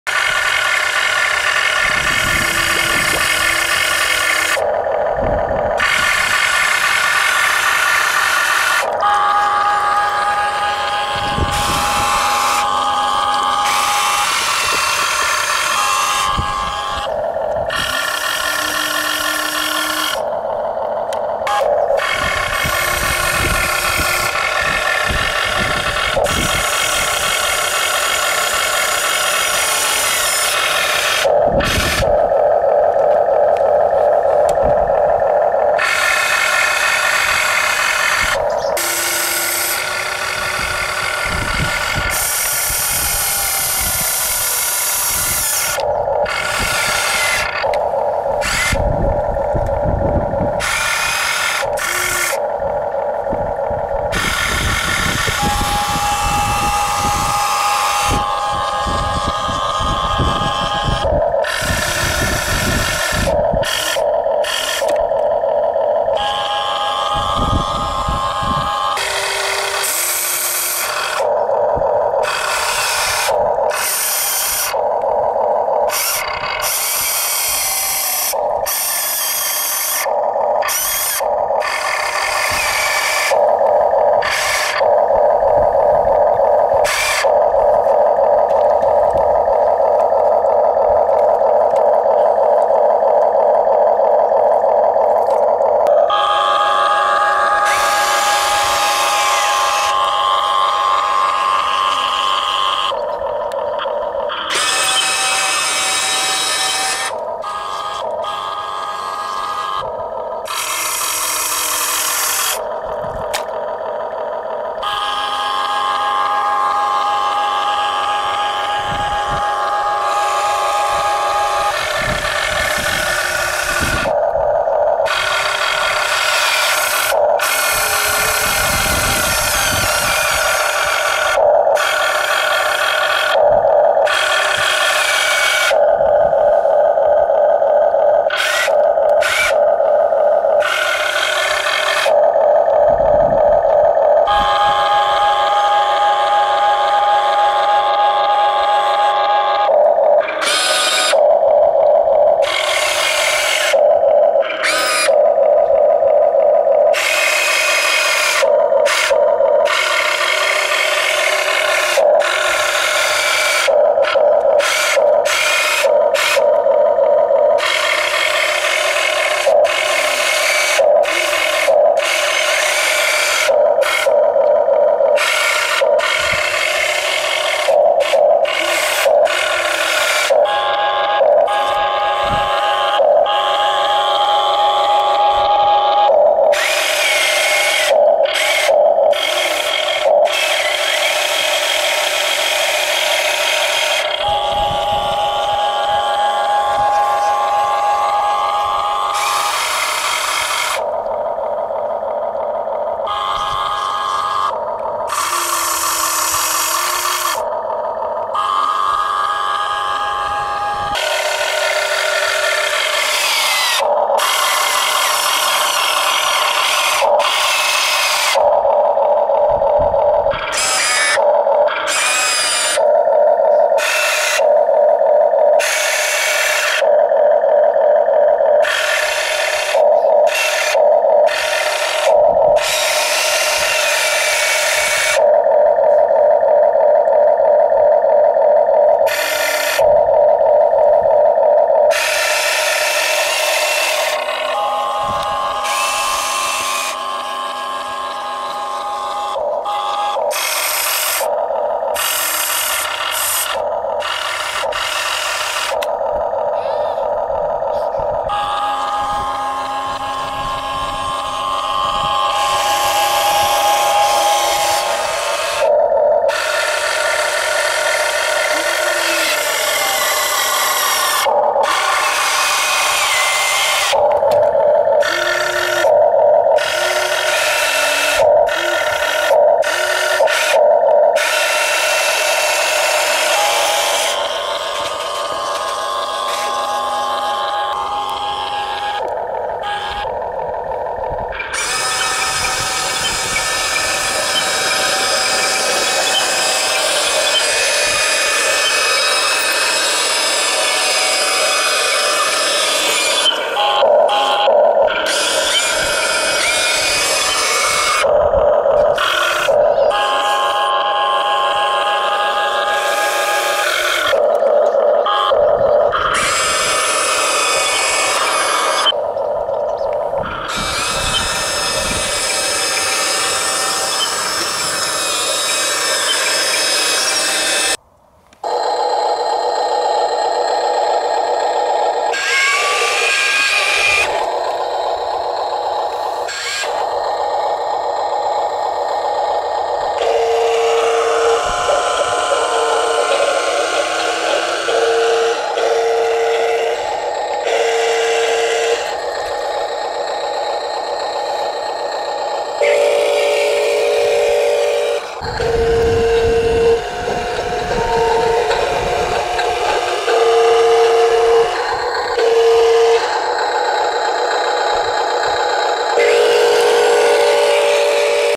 Tractor Excavator and Dump Road Truck at work
Wonderful detailed Excavator RC Action on the mud.